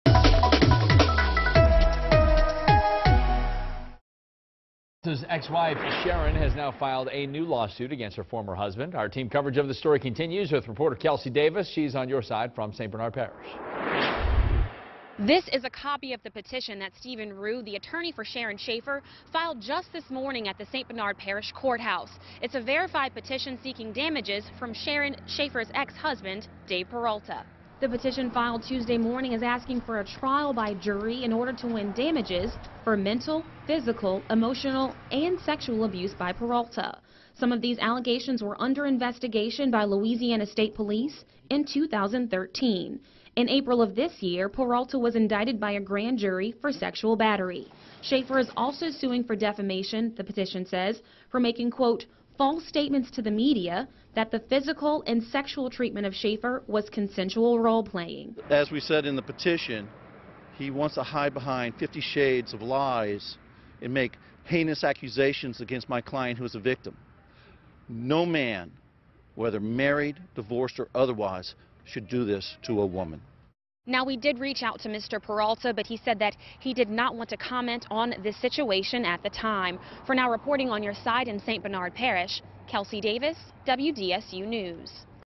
St. Bernard Parish president's ex-wife files new lawsuit
Dave Peralta's ex-wife files a new lawsuit against him.